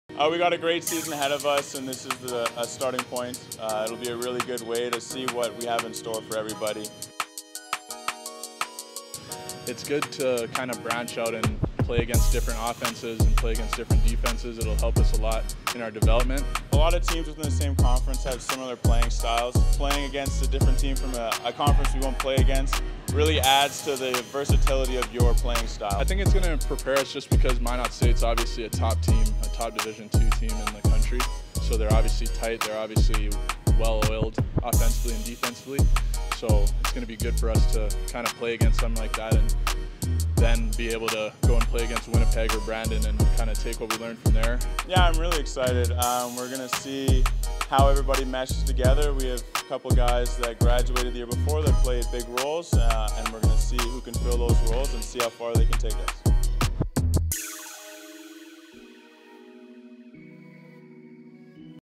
University of Regina Cougars men's basketball game preview vs. Minot State
We chat with veterans Carter Millar and Nigel Warden as the University of Regina's men's basketball team gets set to host Minot State in a pair of exhibition games on August 18 and August 19.